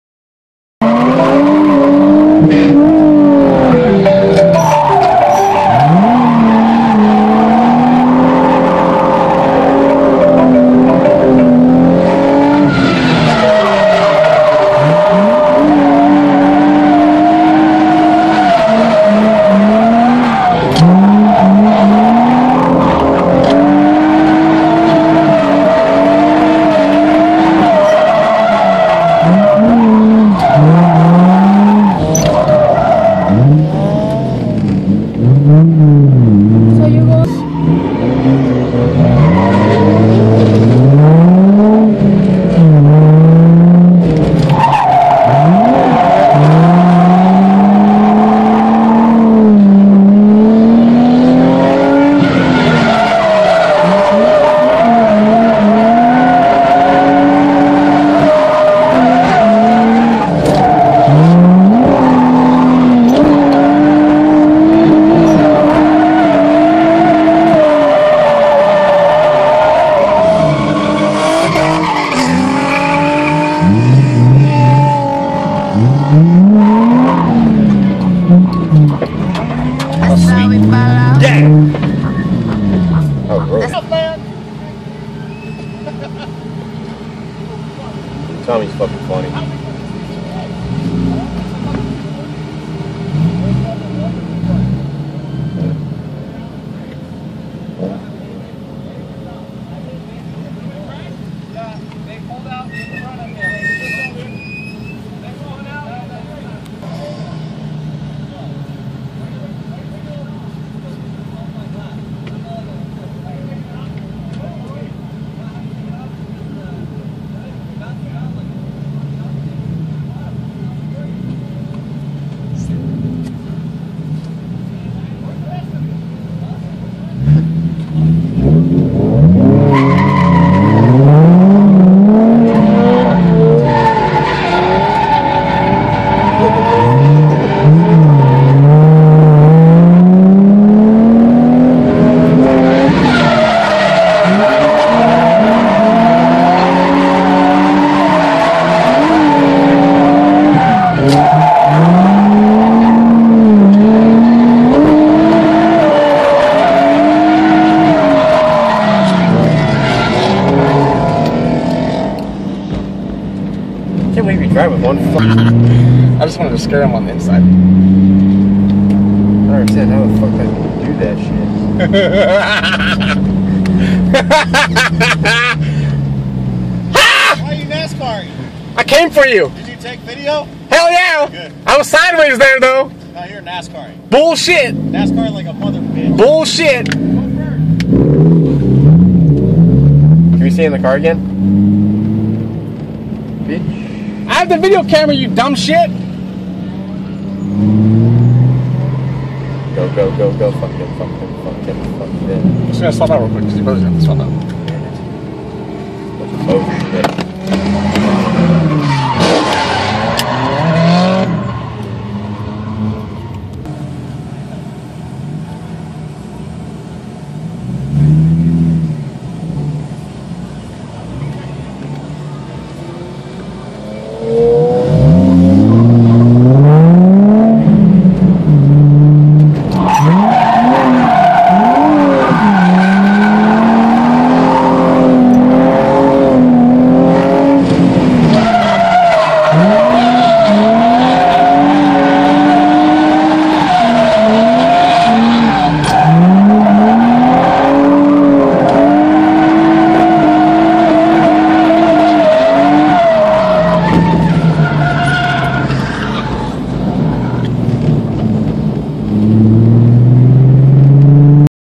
Tandem fun at adams motorsports park 350z and FRS
FRS and 350z tandem 5/30/2013